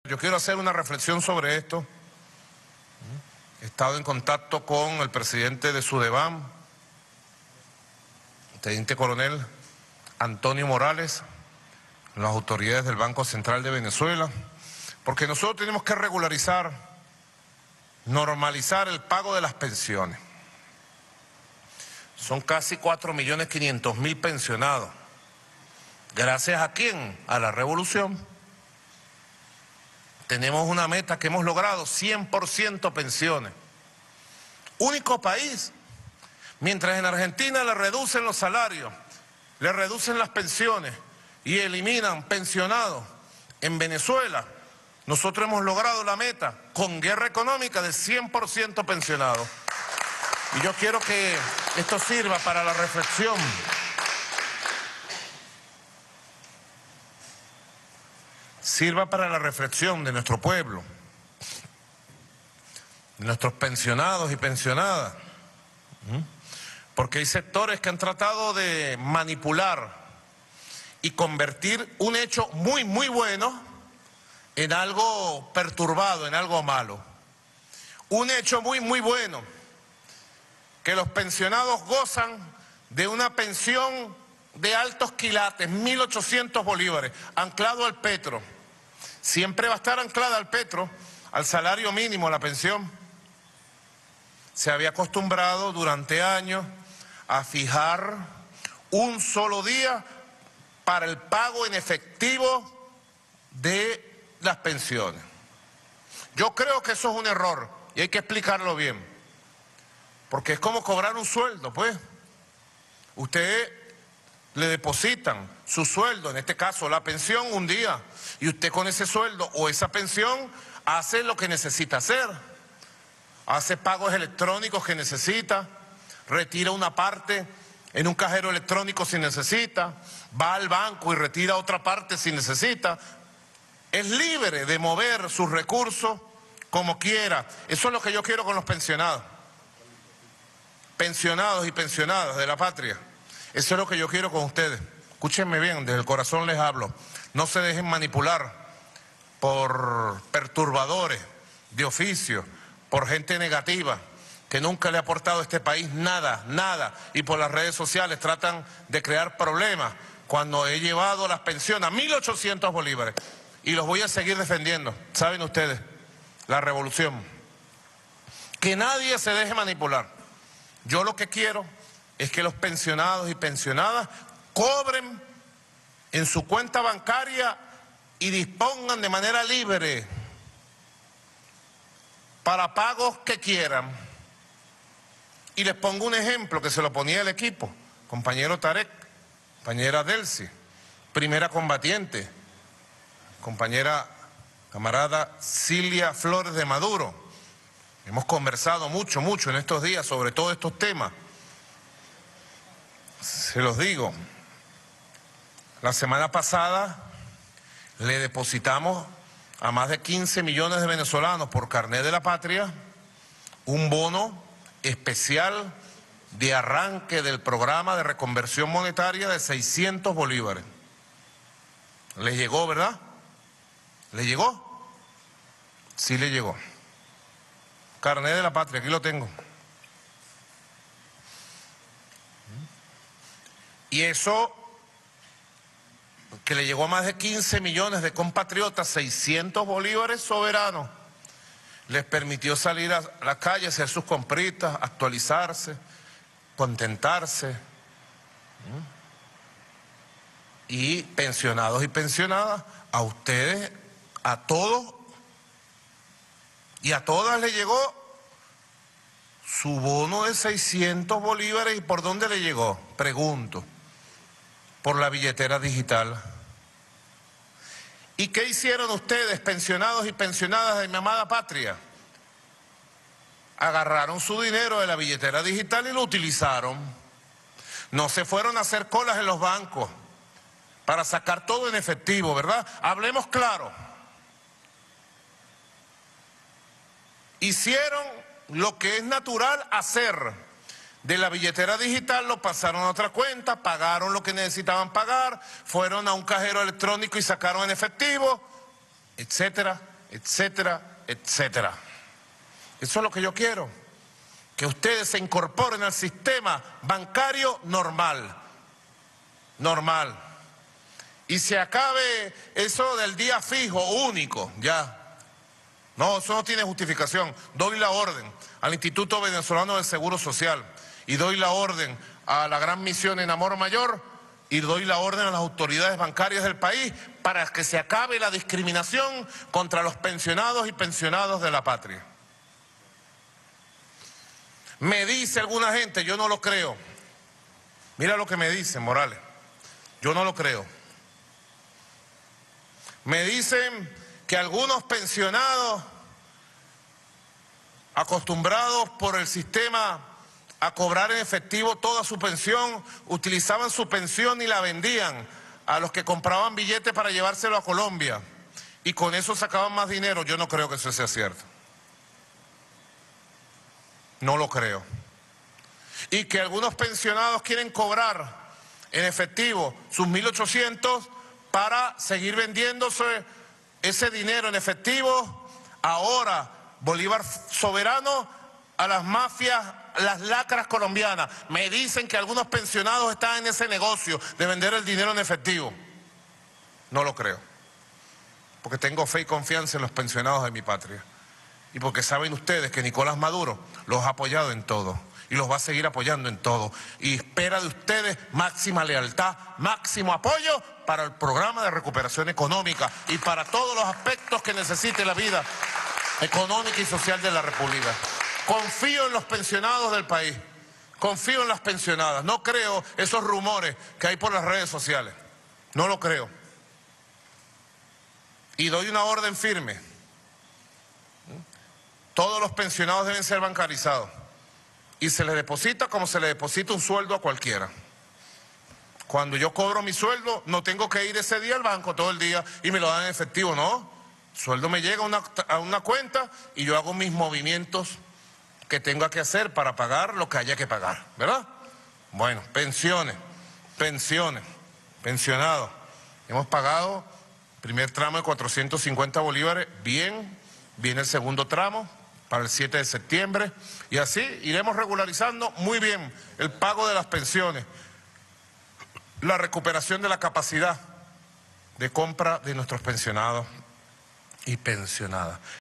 El mensaje de Nicolás Maduro a los pensionados este 3 de septiembre 2018
El Presidente venezolano, Nicolás Maduro, dedica parte de su cadena este 3 de septiembre de 2018 a las y los pensionados, para no dejarse engañar por personas que intentan manipularlos luego de haber subido el salario mínimo y las pensiones de BsS 50 a BsS 1.800